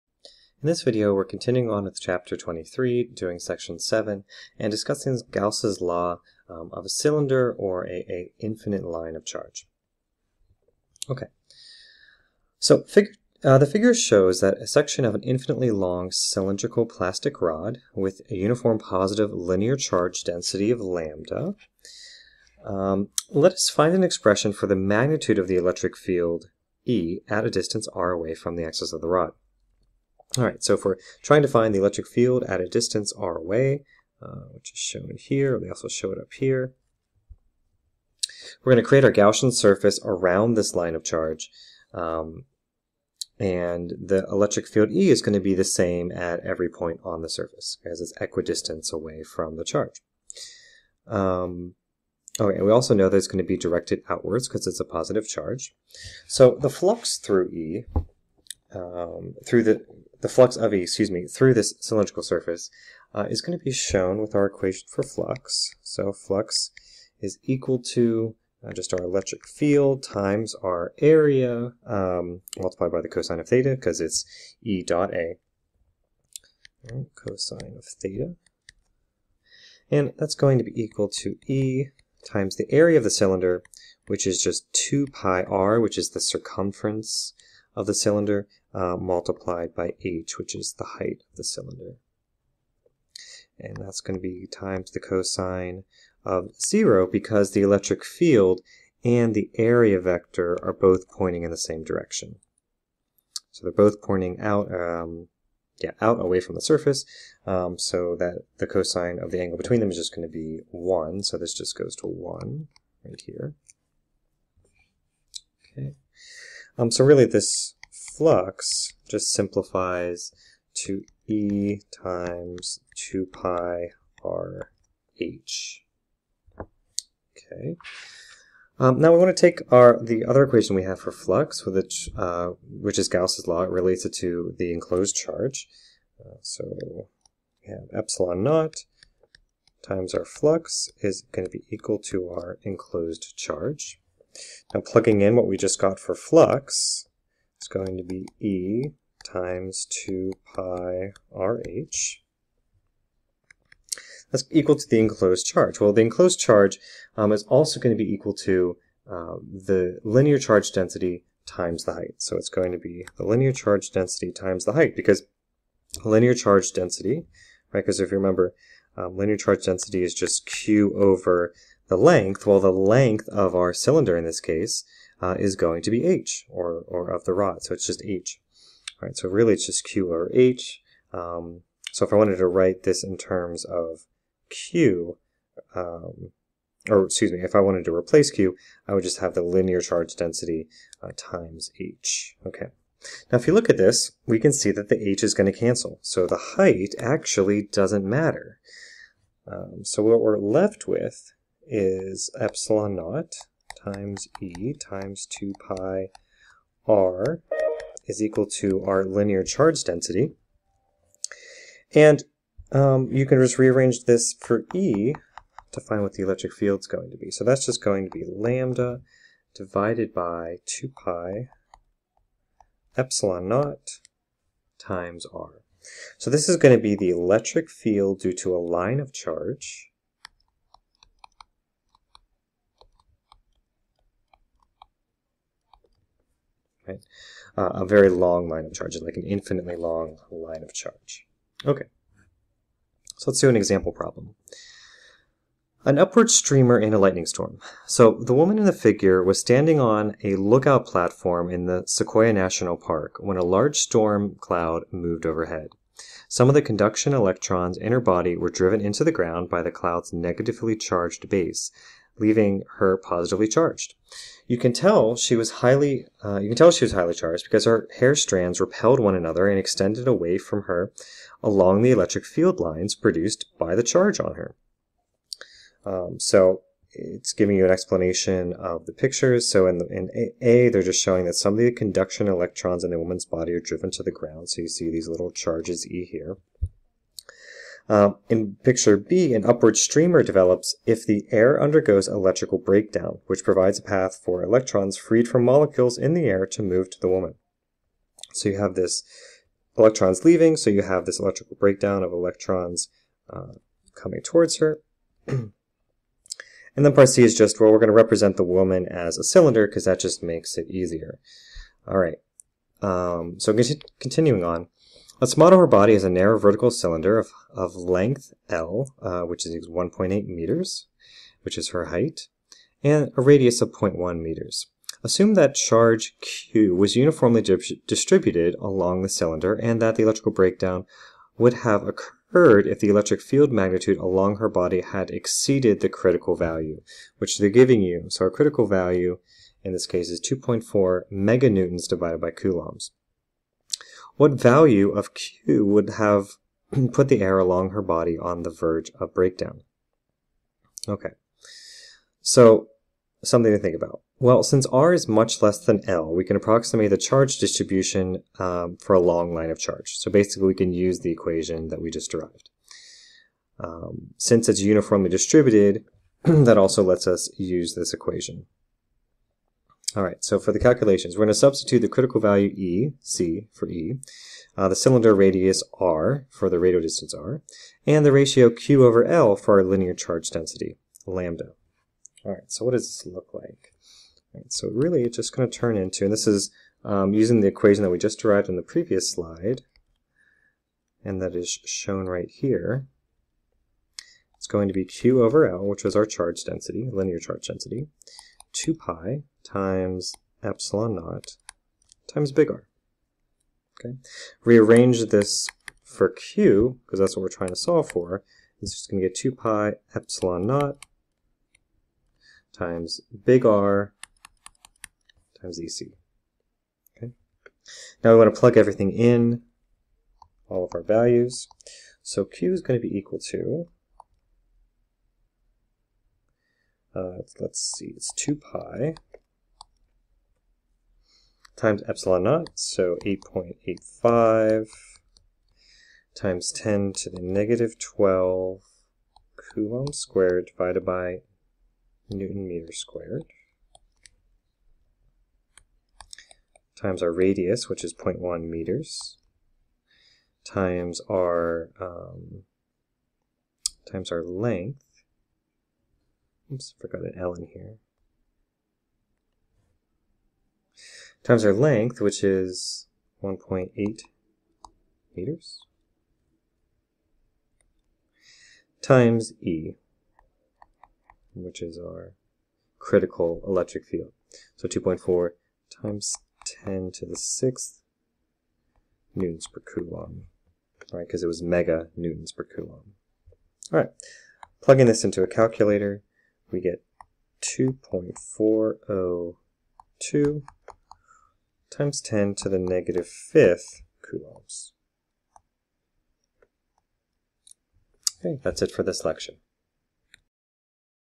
Electricity and Magnetism (Serway 24-3)
AP Physics C: Electricity and Magnetism
Gauss' Law
Chapter 23.7
Infinite Line of Charge